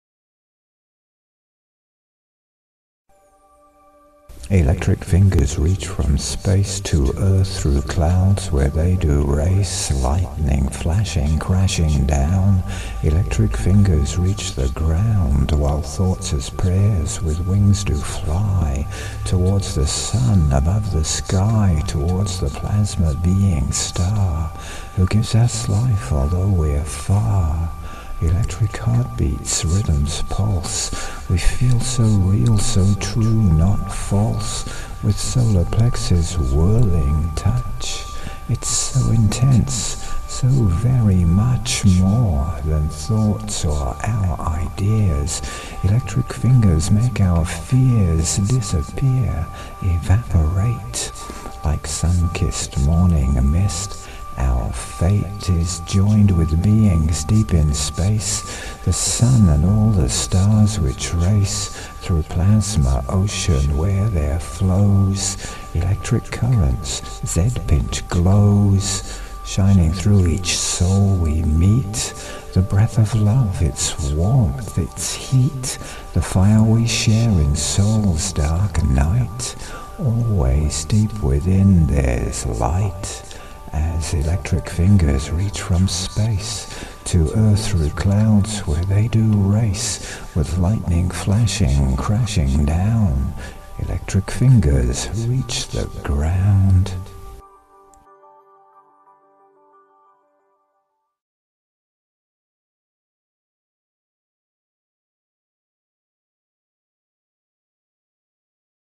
dark night
New version of 2006 poem by Lawrence Hearn, Poet Laureate of the Electric Universe, at BCPOETRY.CA